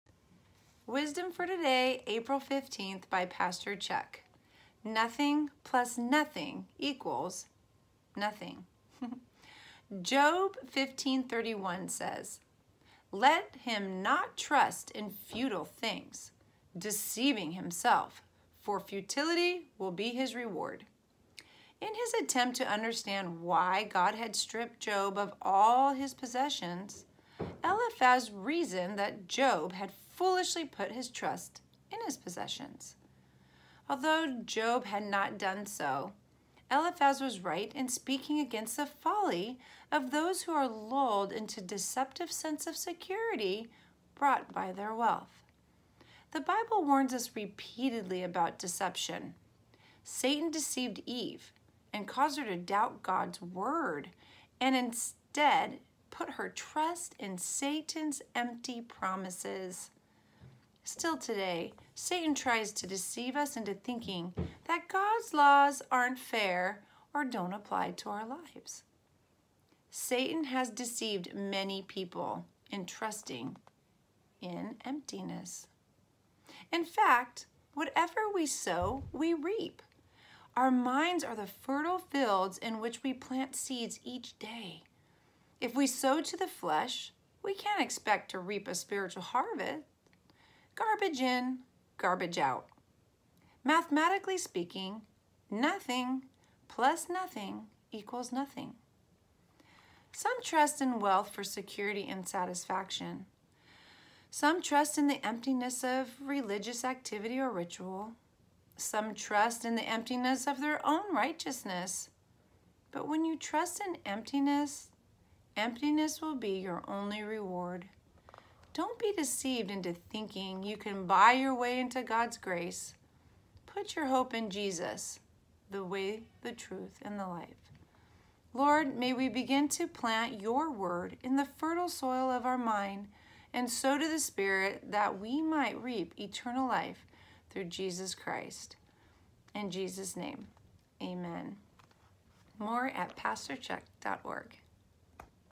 April 15-Wisdom for Today
Nothing + Nothing = Nothing
Job 15:31

Read by
Vanessa (granddaughter)